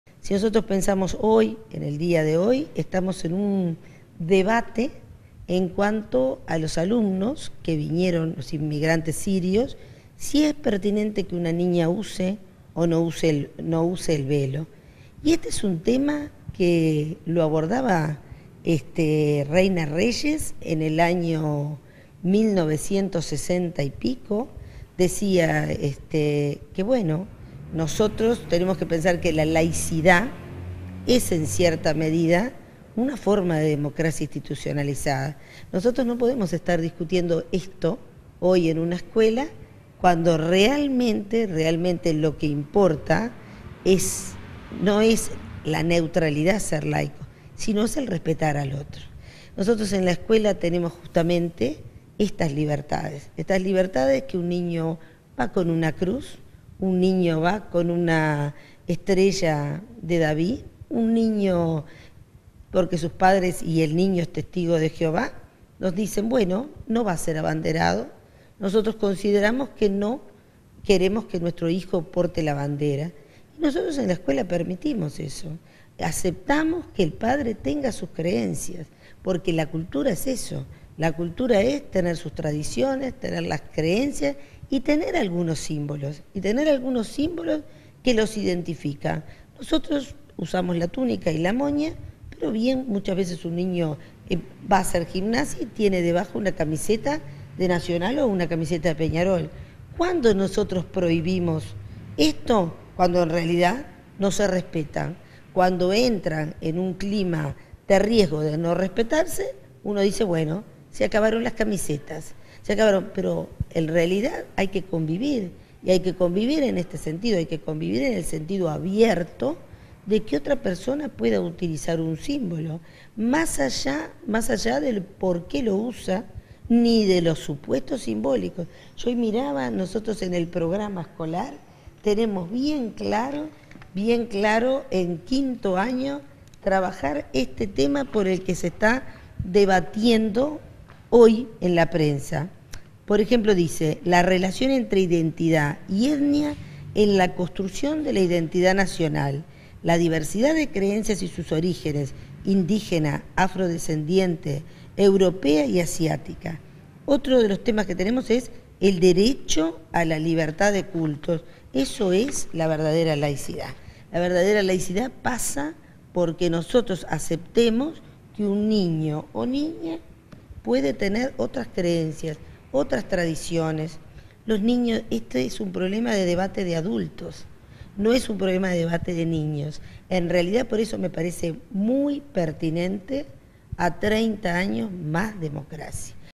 La Directora del CEIP opina sobre el uso del velo en las escuelas públicas.
Entrevistada por la Secretaría de Comunicación, Irupé Buzzetti, Directora General del Consejo de Educación Inicial y Primaria,  dijo que "la laicidad es en cierta medida una forma de democracia institucionalizada (...) realmente lo que importa no es la neutralidad de ser laico, sino es el respetar al otro".